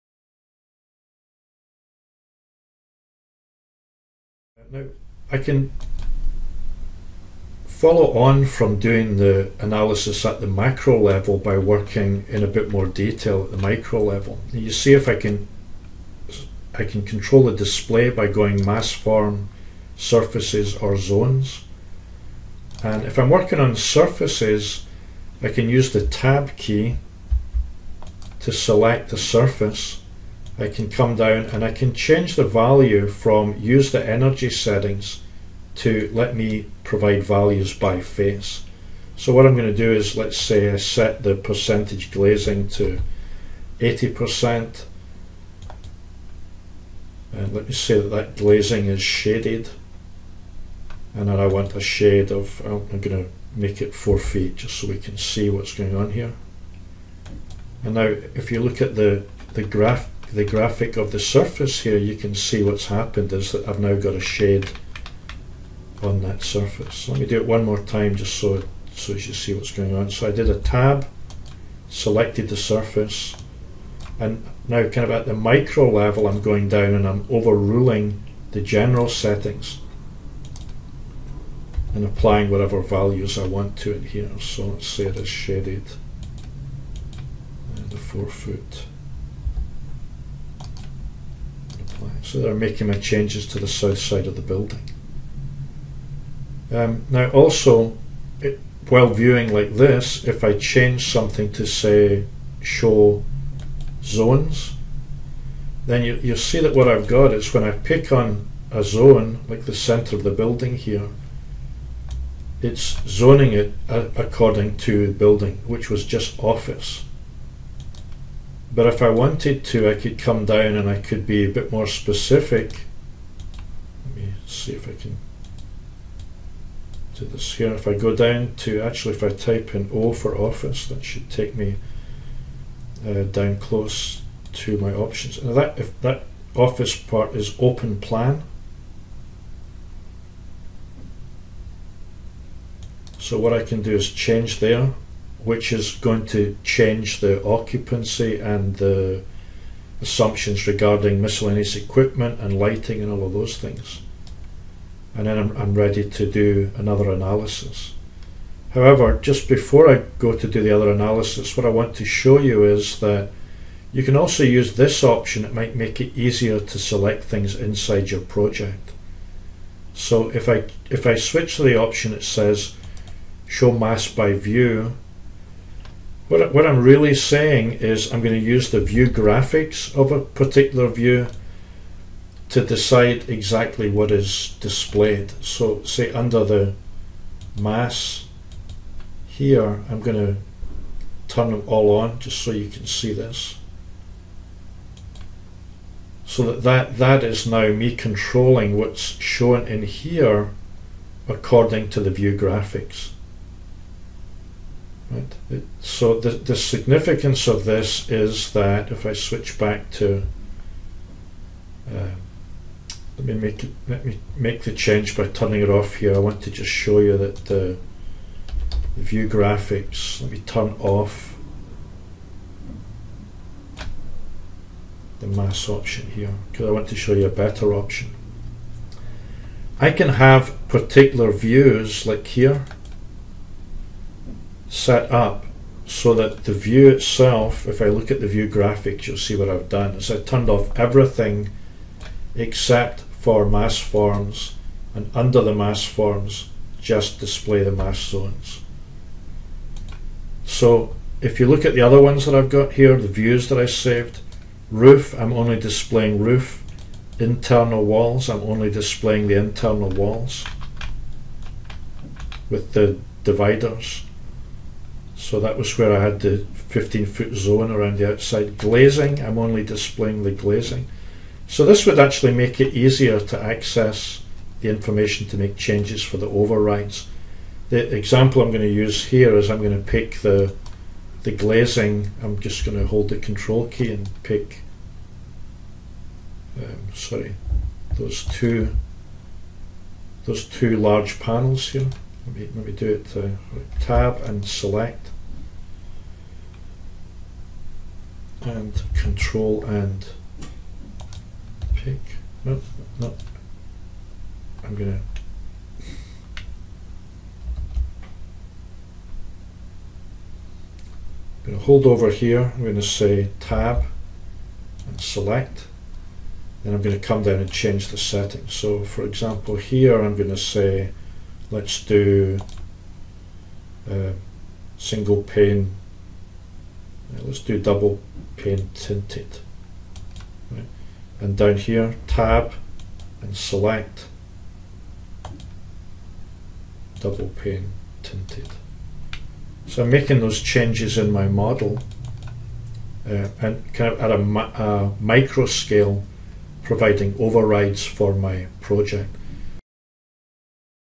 Changing Energy Settings: Zones, Surfaces, and Glazing
How to change the energy settings of specific zones and surfaces in your energy model. Including how to isolate building features like glazing, roofs, and walls for easier editing.